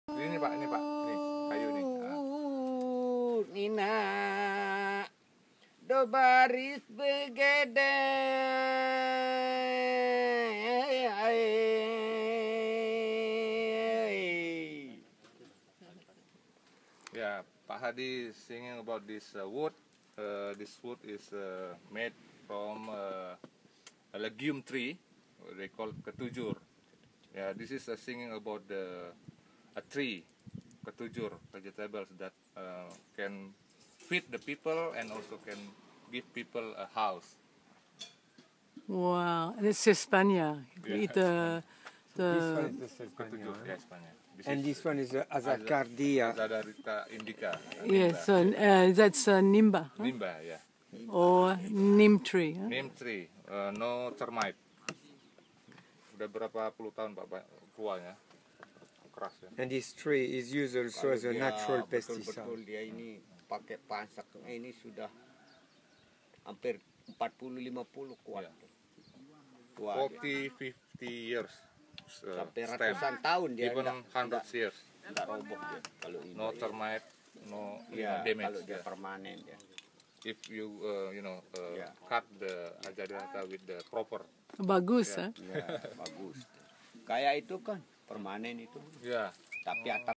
Song about Sesbania Tree
Bapu Sadi sings about the Sesbania grandiflora tree that he used to construct the struts in his shelter on Pantai Pink (Pink Beach), Tanjung Ringgit, the peninsula forming south eastern Lombok, Indonesia. Its young leaves are sold in bundles as a leafy vegetable, the large white flower buds and tender fruit pods are eaten in salads, curries and soups.
The posts are made of Neem timber (Azadirachta indica) that is termite resistant for even up to a hundred years. We saw many neem trees, of all sizes up to seven metres growing on Tanjung Ringgit.